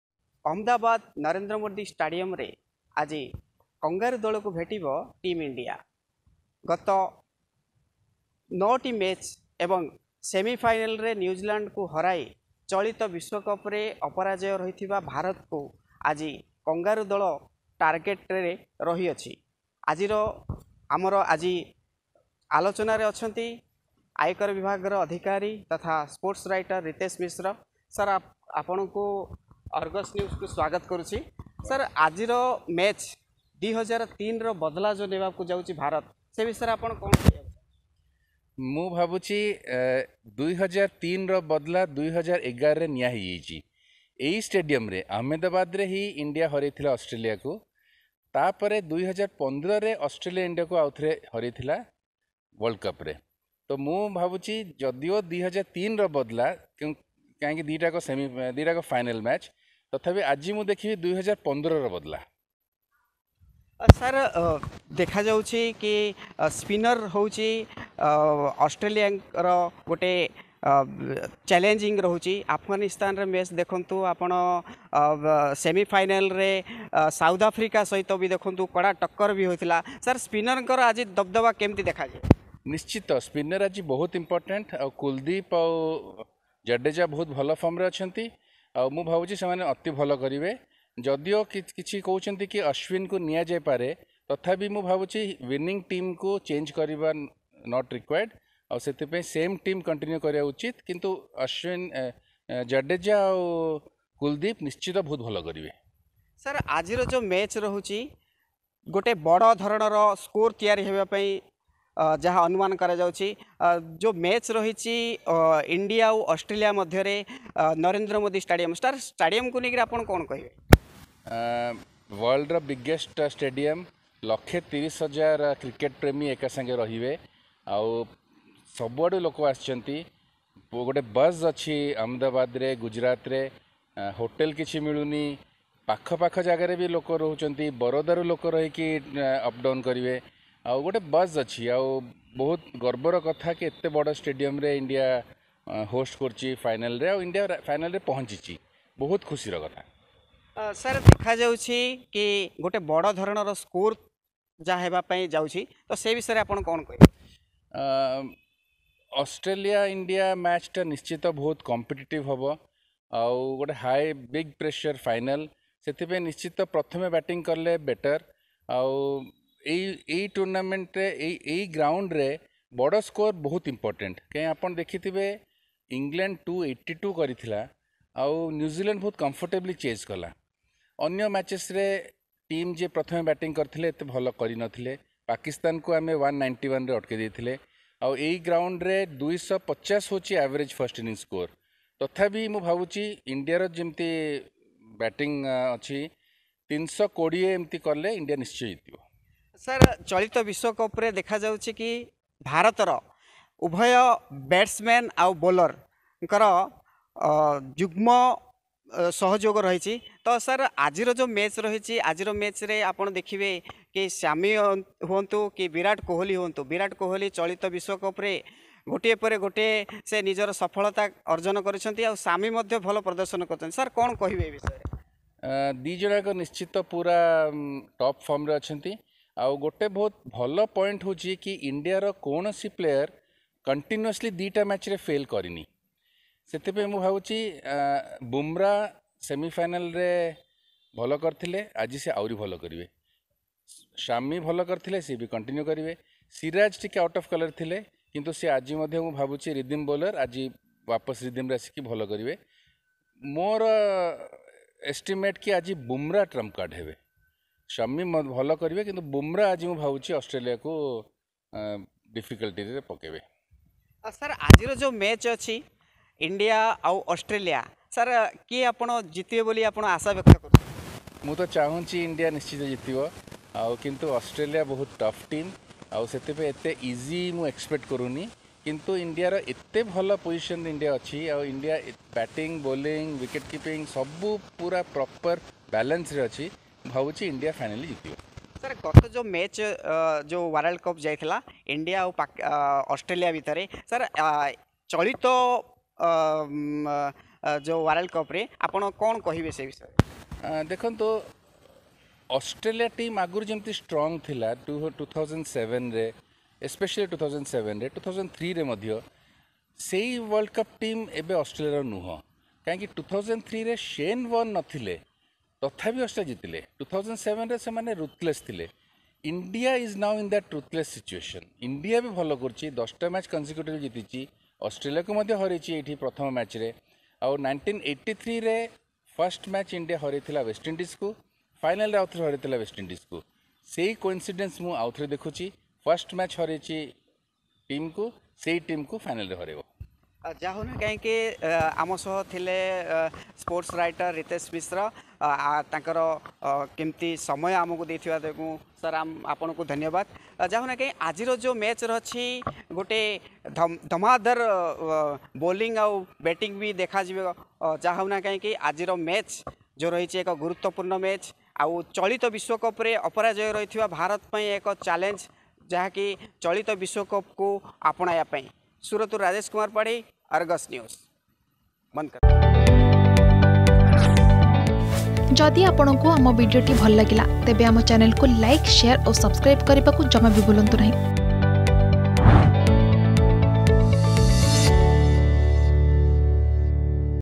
ଆଜି ୨୦୦୩ ସହ ୨୦୧୫ର ପ୍ରତିଶୋଧ ନେବ ଭାରତ || World Cup Final || India vs Australia
ଆଜି ୨୦୦୩ ସହ ୨୦୧୫ର ପ୍ରତିଶୋଧ ନେବ ଭାରତ: ରିତେଶ ମିଶ୍ର, ସ୍ପୋର୍ଟସ୍ ରାଇଟର ।